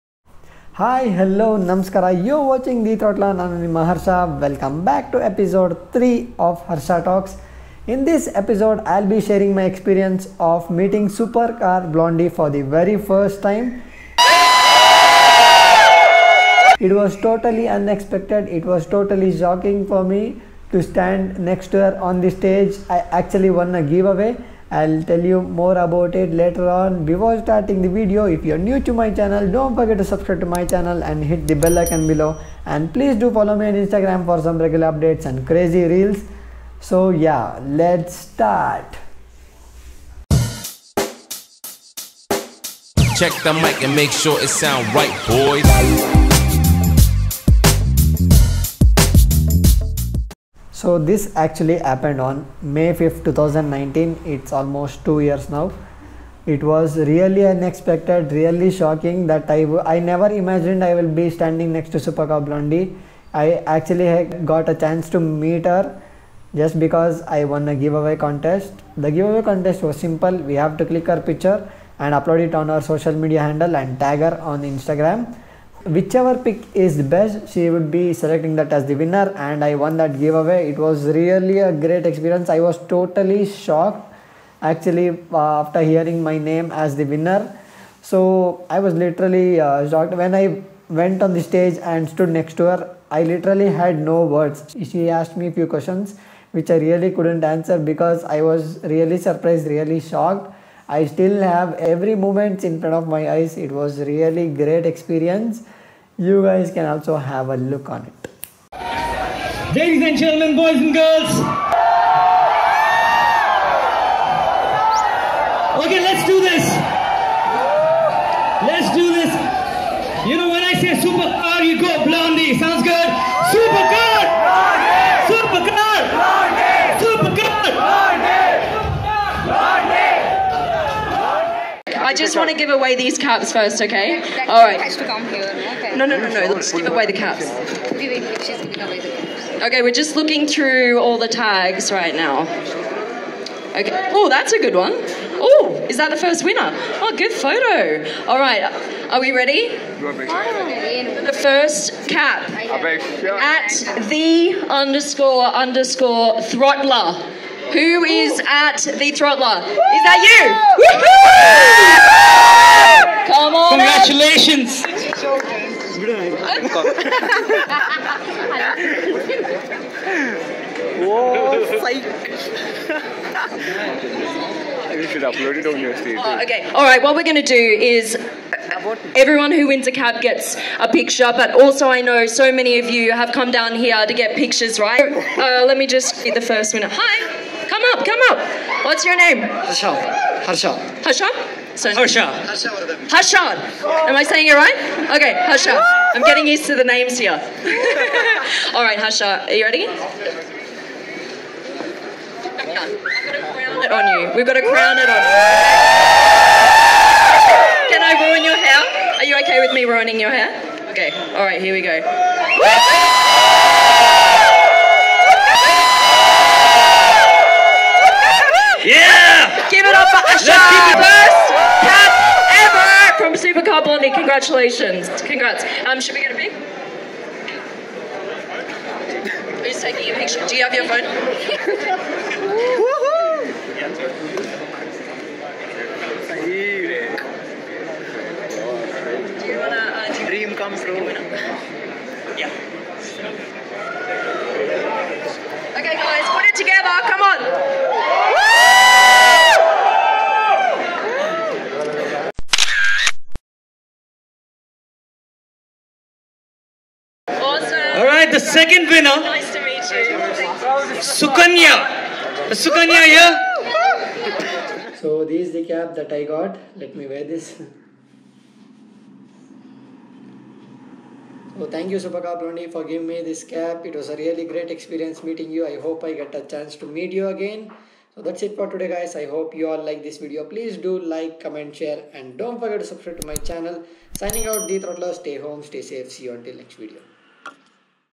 Supercar Blondie just gave me a Cap | Supercar blondie in india | Harsha Talks Episode 3
This was a dream come true, it was such a overwhelmed experience that i had meeting supercar blondie when she visited india back then in 2019, Thank you supercar blondie

See You Untill next Video